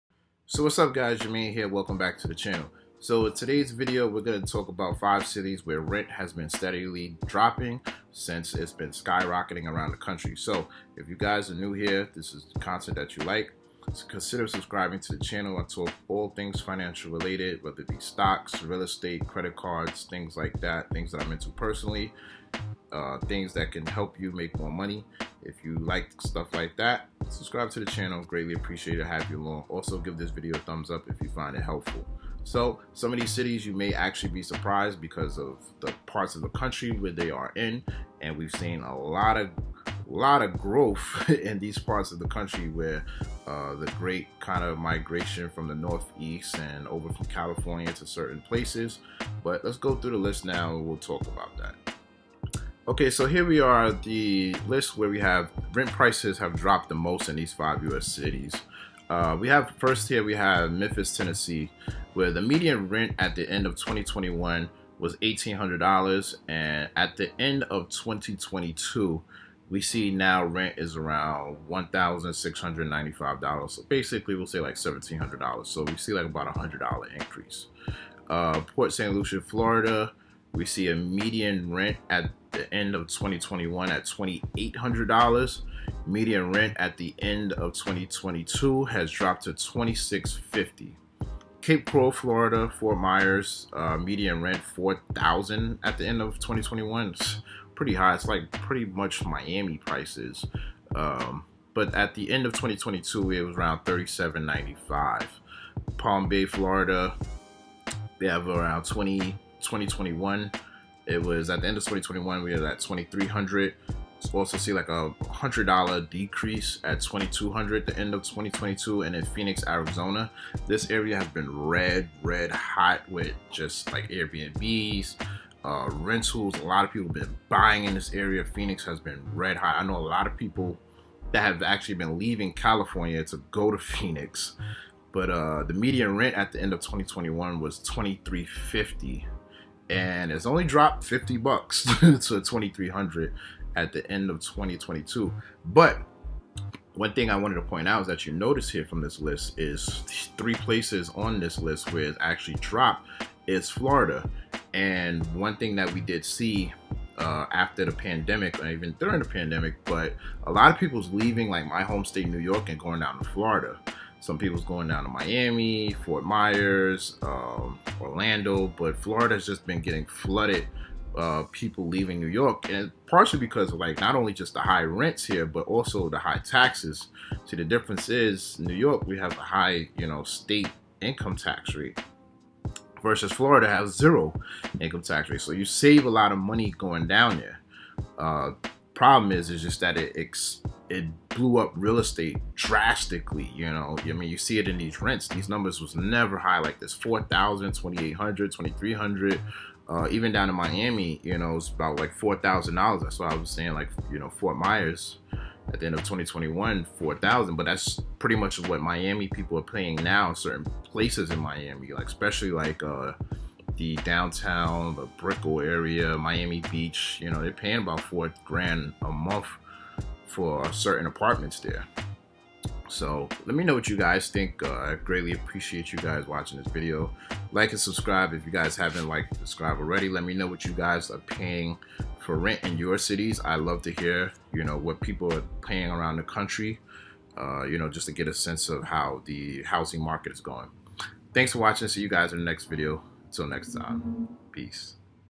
Rent Prices Dropped In These Cities
💰Join Webull with my link and we'll both get free stock 👉🏾

Rent prices have
dropped the most in
these 5 U.S. metros
1 Memphis, TN
Median rent at the end of 2021: $1,800
Median rent at the end of 2022: $1,695
2 Port St. Lucie, FL
Median rent at the end of 2021: $2,800
Median rent at the end of 2022: $2,650
3 Cape Coral, FL; Fort Myers, FL
Median rent at the end of 2021: $4,000
Median rent at the end of 2022: $3,795
4 Palm Bay, FL; Melbourne, FL; Titusville, FL
Median rent at the end of 2021: $2,300
Median rent at the end of 2022: $2,200
5 Phoenix, AZ; Mesa, AZ; Chandler, AZ
Median rent at the end of 2021: $2,350
Median rent at the end of 2022: $2,300
Source: HouseCanarv
CNBC